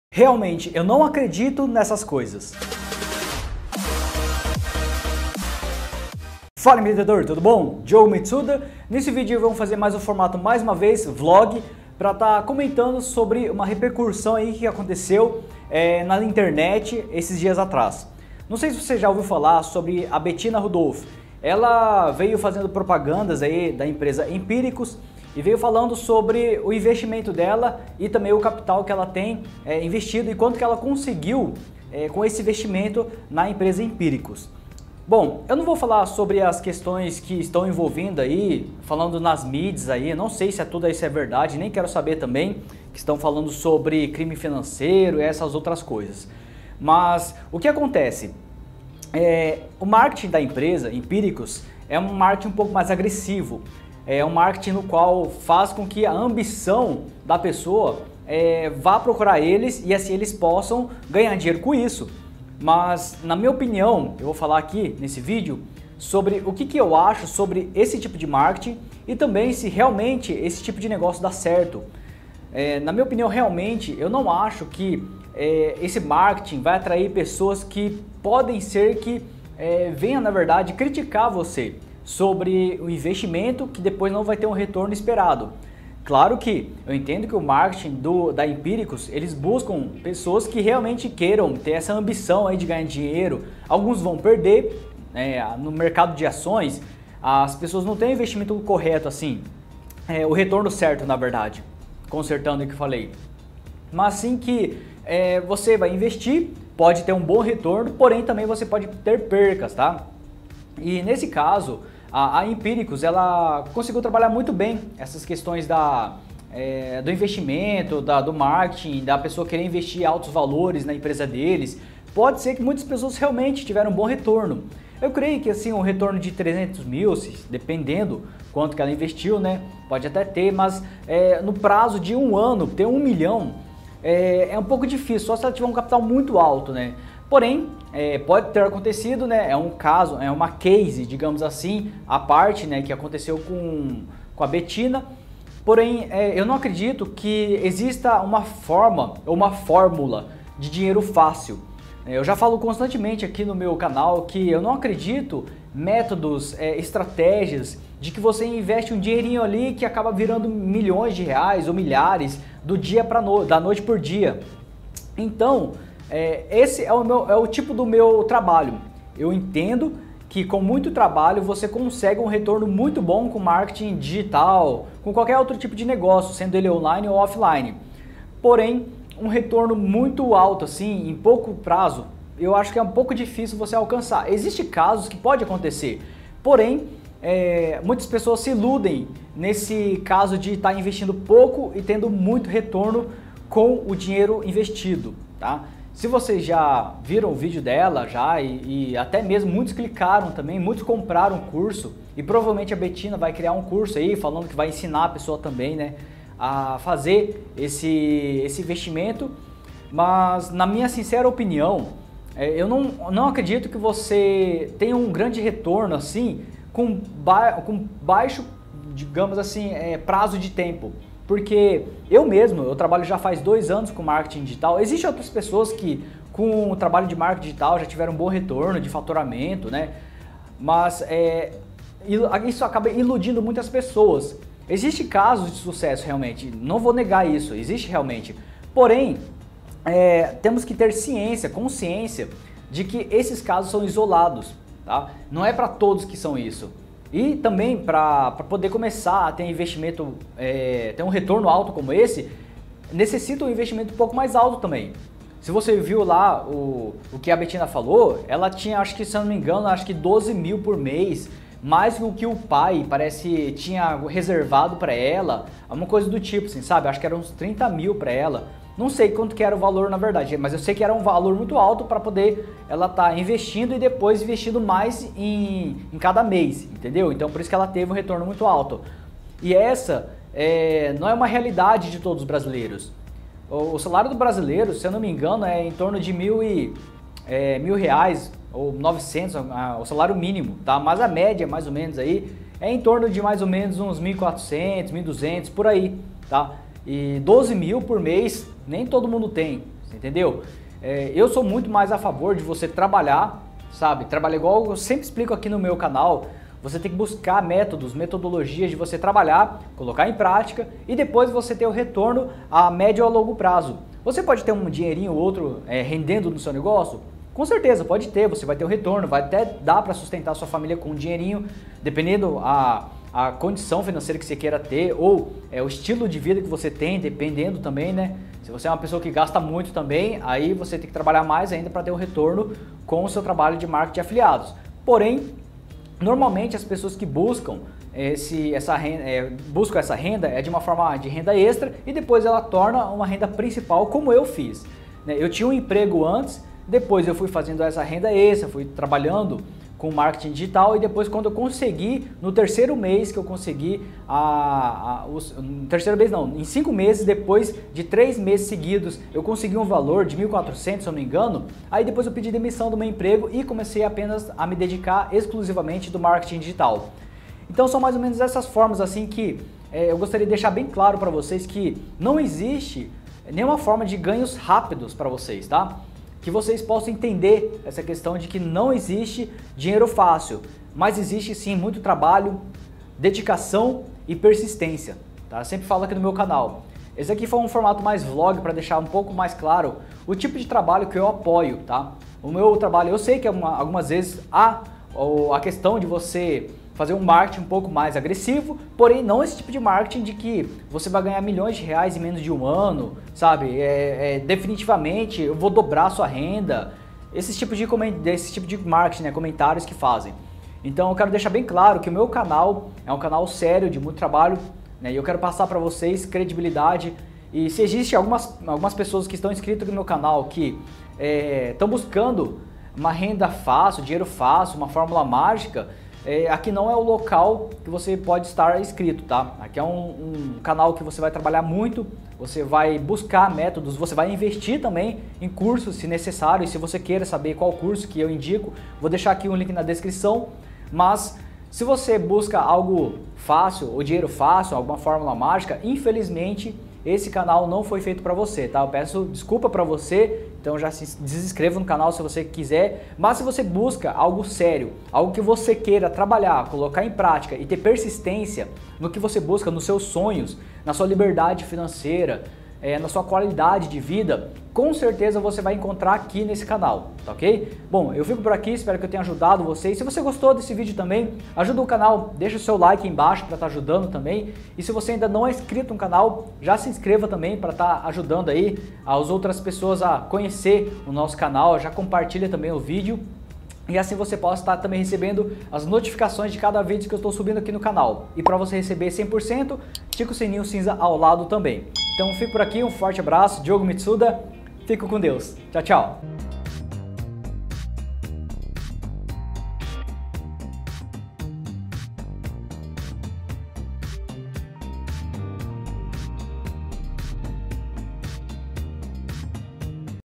Bettina Rudolph Minha Opinião Sincera | Diogo Mitsuda
👉 Acesso Mini Curso Gratuito!

Neste vídeo venho falar sobre a minha opiniãos obre o patrimônio construido pela Empiricus através da Bettina.
Não irei entrar no mérito de crimes financeiros, se está certa ou errada.
Mas apenas irei falar o que é a minha opinião sobre esse “caso de sucesso”.

👉 Meu Curso Grátis!

Forte abraço!
Sucesso!

Diogo Mitsuda

PESQUISAS COMUNS SOBRE:

➡️️ renda extra
➡️️ trabalhar em  casa
➡️️ como ganha dinheiro
➡️️ ganhar dinheiro na internet
➡️️ eduzz